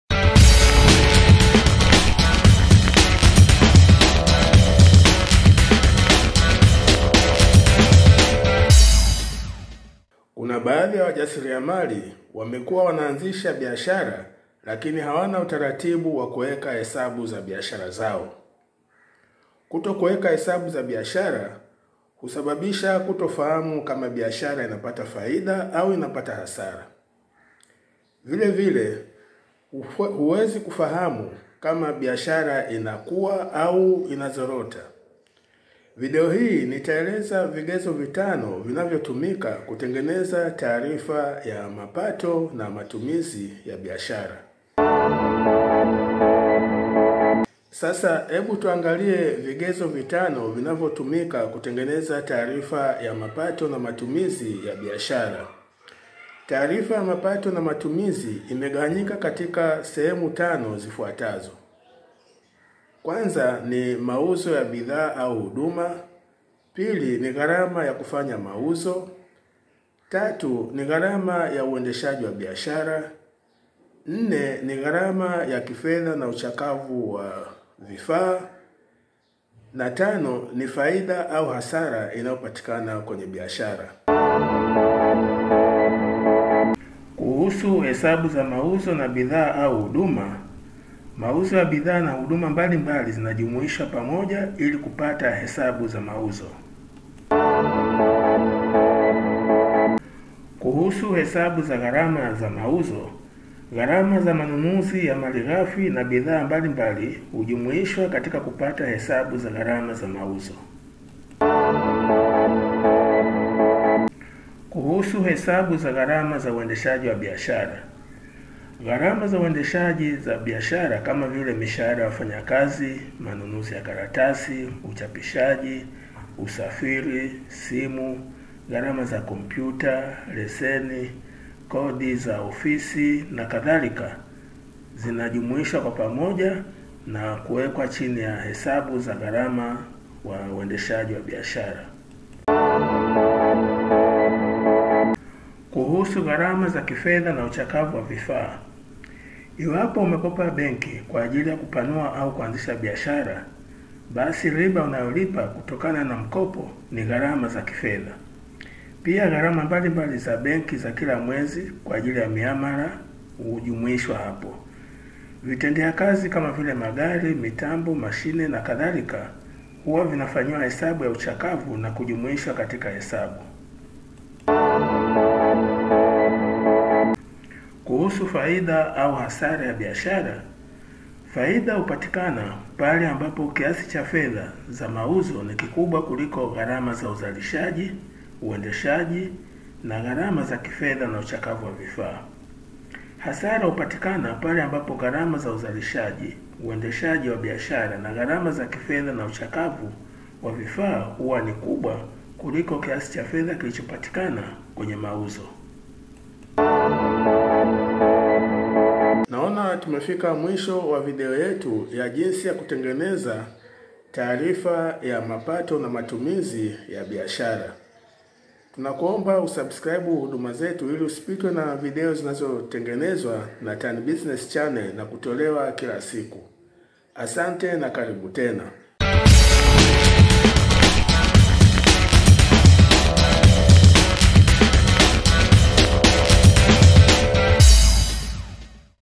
Jinsi ya kutengeneza taarifa ya mapato na matumizi ya biashara
Kuna baadhi ya wajasiriamali wamekuwa wanaanzisha biashara, lakini hawana utaratibu wa kuweka hesabu za biashara zao.

Kutokuweka hesabu za biashara husababisha kutofahamu kama biashara inapata faida au hasara.

Vile vile huwezi kufahamu kama biashara inapanuka au inazorota

Video hii nitaeleza vigezo 5 ambavyo hutumika kutengeneza taarifa ya mapato na matumizi ya biashara

Pata mafunzo ya ujasiriamali kupitia Tan Business Online School kupitia link ya